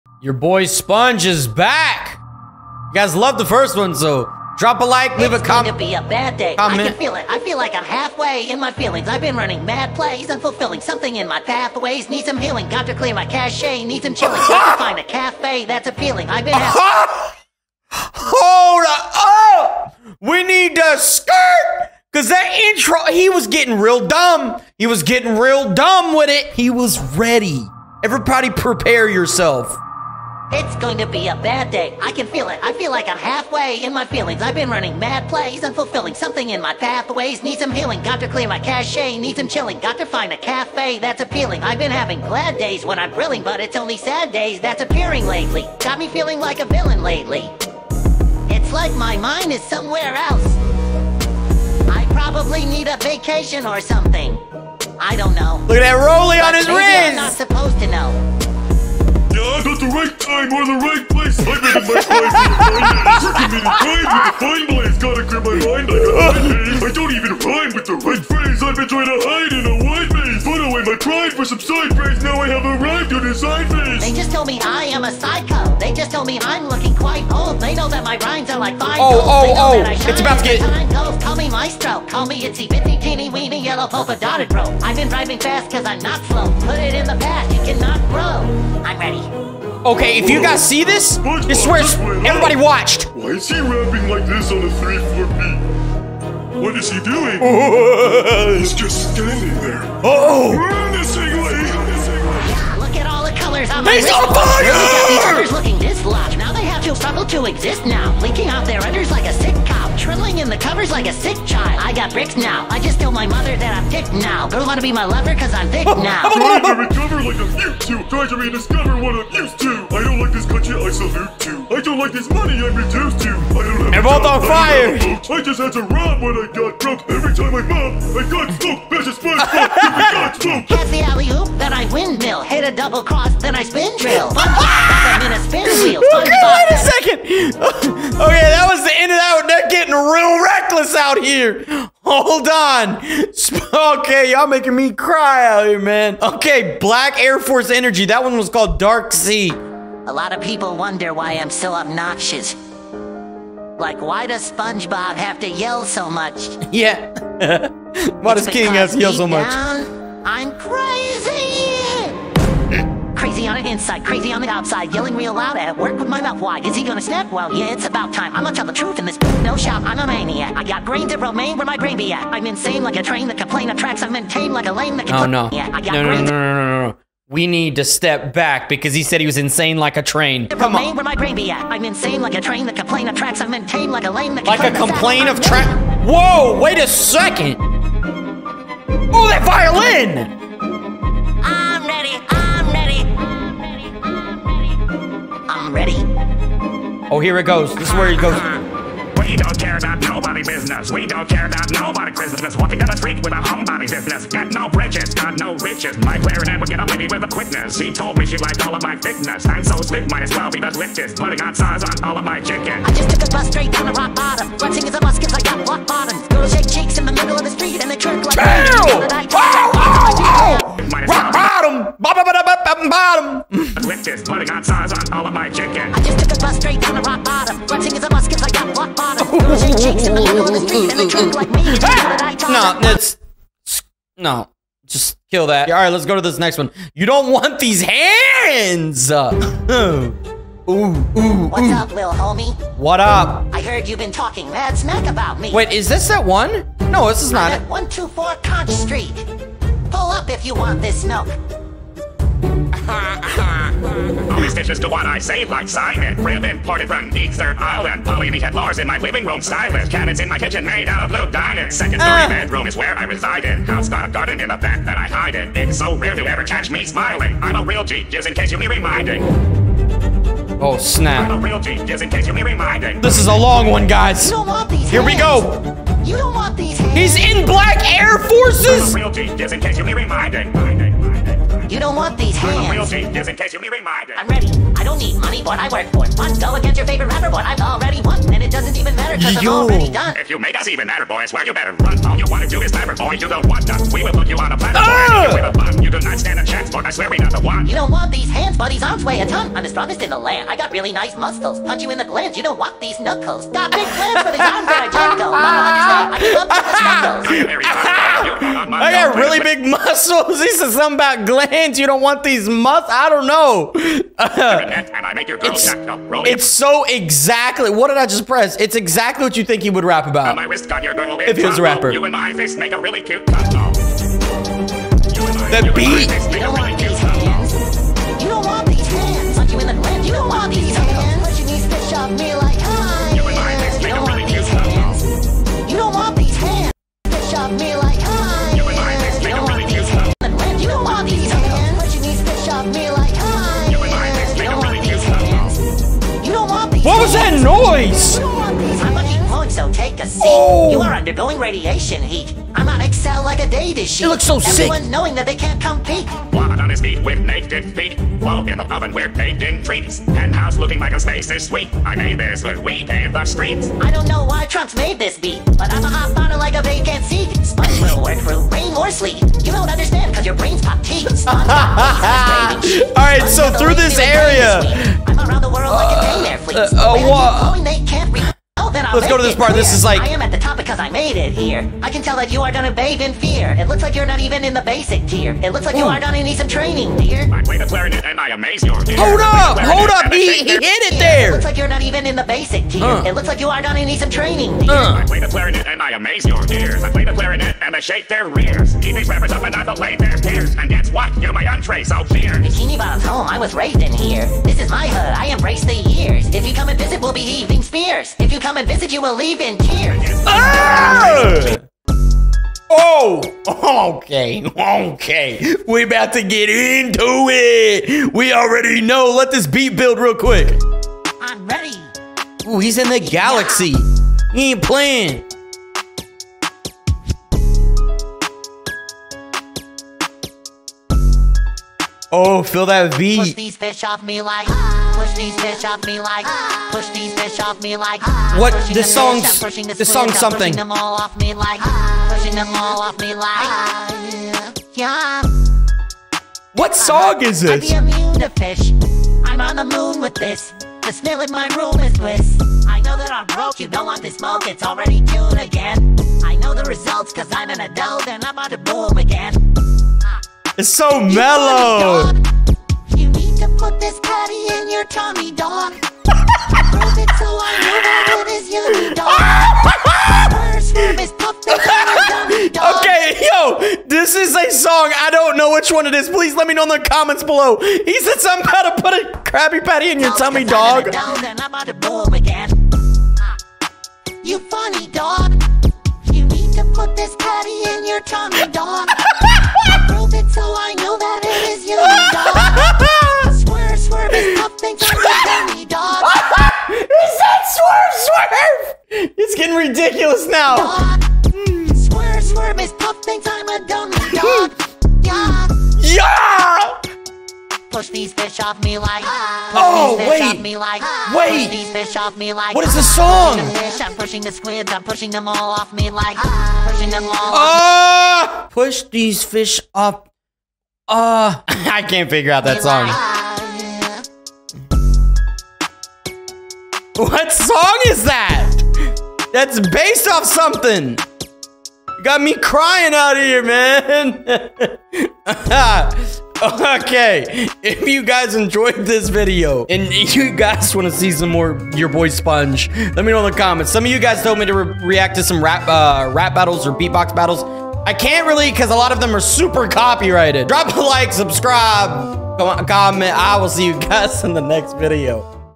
Reacting to GANGSTER SPONGEBOB Rap Songs! (Squidward, Patrick)
Reacting to Gangster Spongebob Rap Songs with Patrick and Squidward!